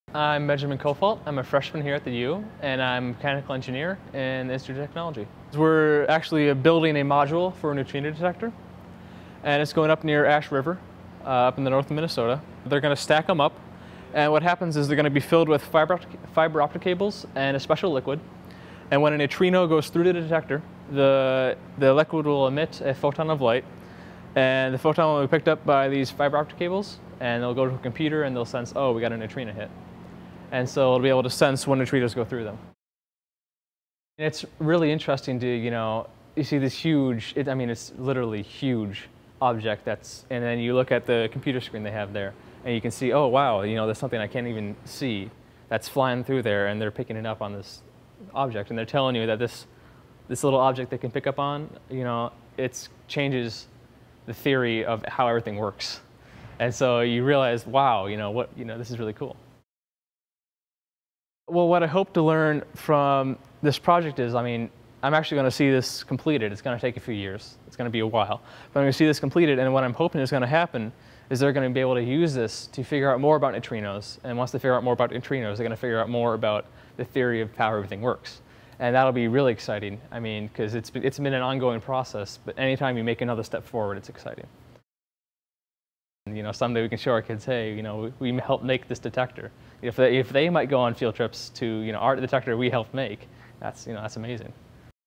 Meet Physics Freshman Ben Kofalt
University of Minnesota students get to work on a wide range of interesting projects that help enhance their educational experience. Freshman Ben Kofalt talks about the work he does in the Physics department that goes hand in hand with the NoVa lab project in Northern Minnesota.